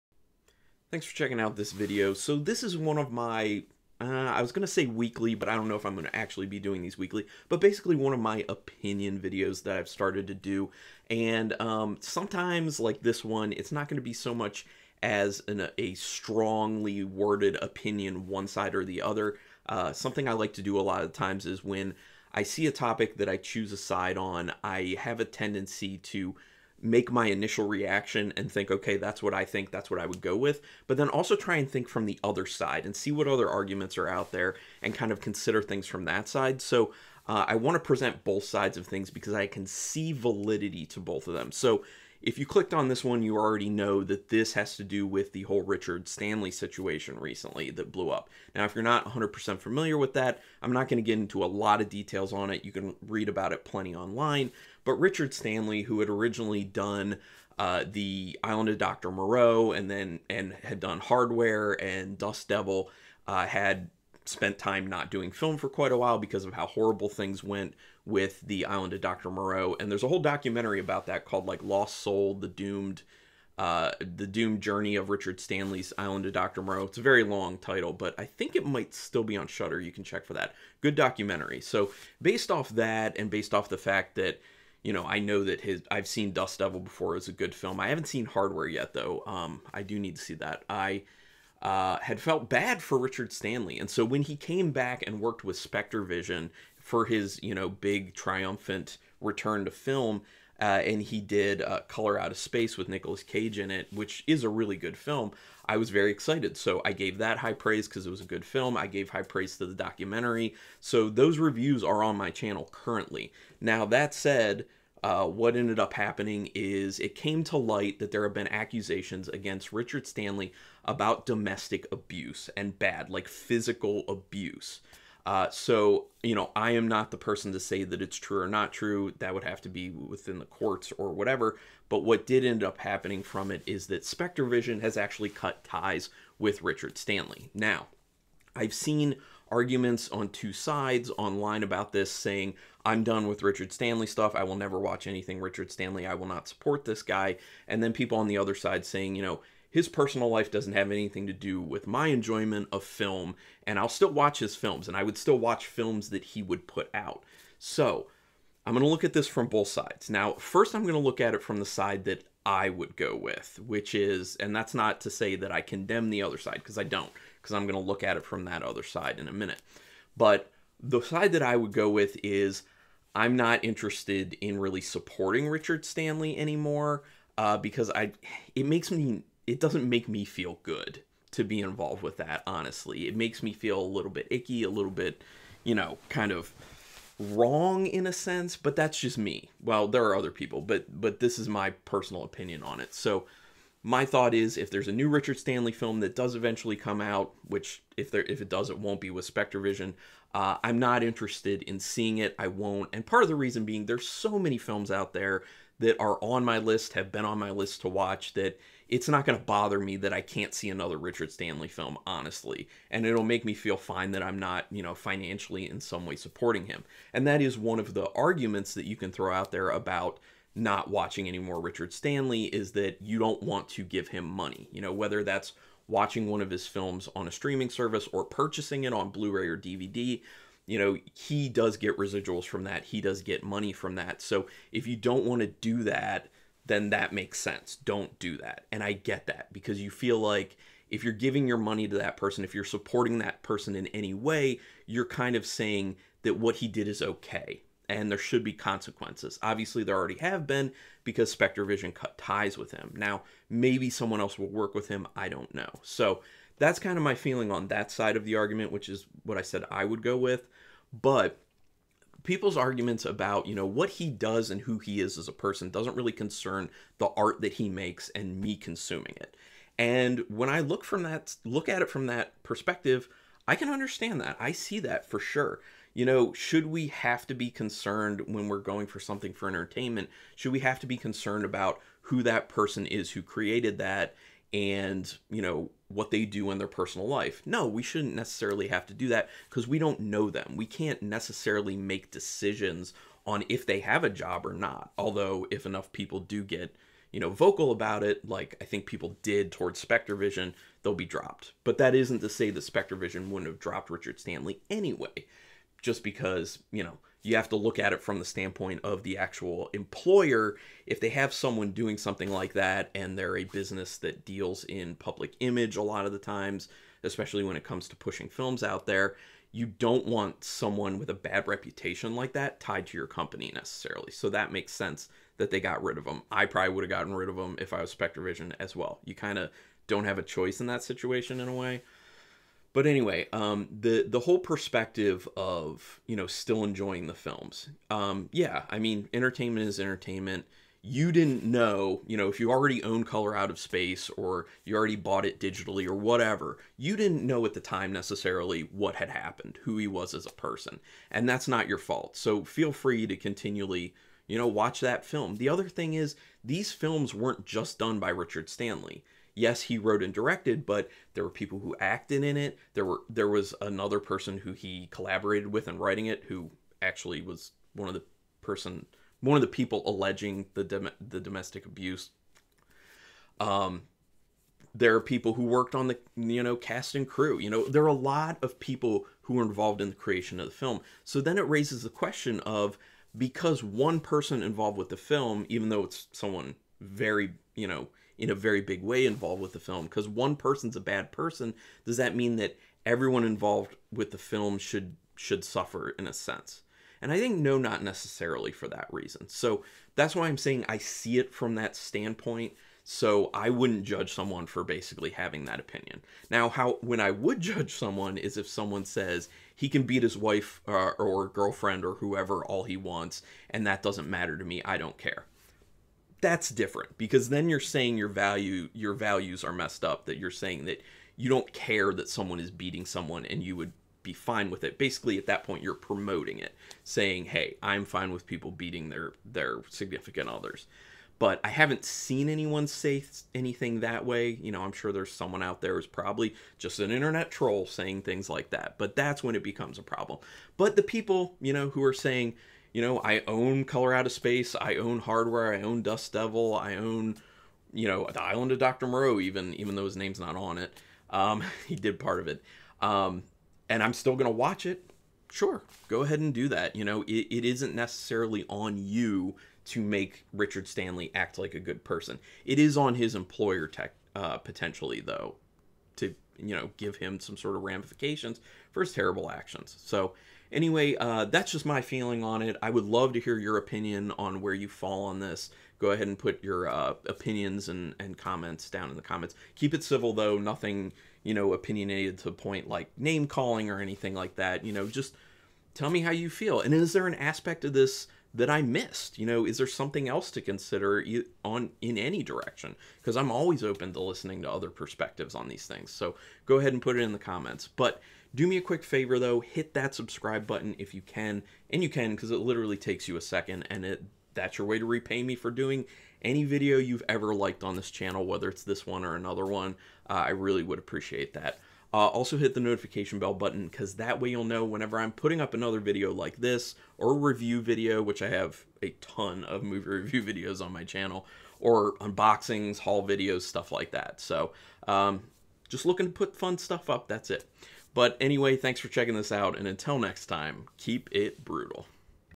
My Reaction To The Richard Stanley Allegations
Carlin briefly talks about the recent allegations of abuse against director Richard Stanley and how people are choosing to react to it.